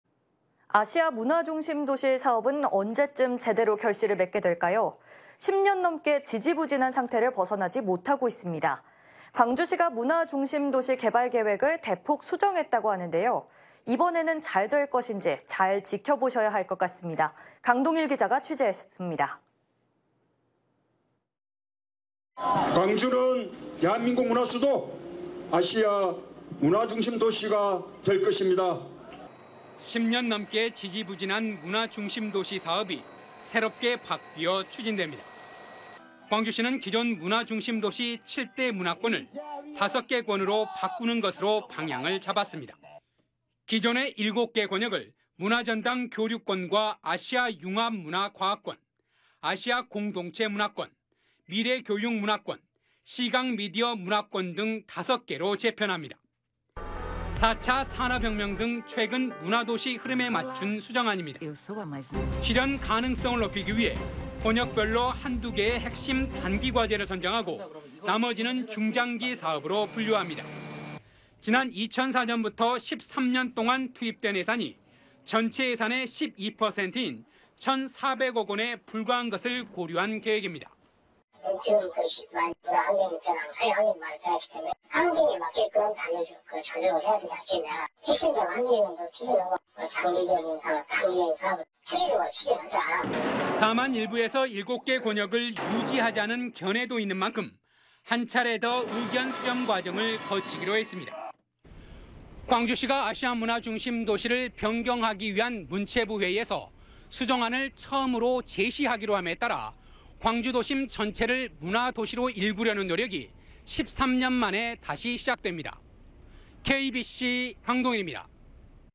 멈춘 7대 문화권 수정안 윤곽.. 5개 권역 재편 유력
아시아 문화중심도시 사업은 언제쯤
제대로 결실을 맺게 될까요?

10년 넘게 지지부진 한 상태를 벗어나지
못하고 있습니다.

광주시가 문화중심도시 개발 계획을
대폭 수정했다고 합니다.

이번에는 잘 될 것인지 잘 지켜 보셔야 할
것 같습니다.

강동일 기자가 취재했습니다.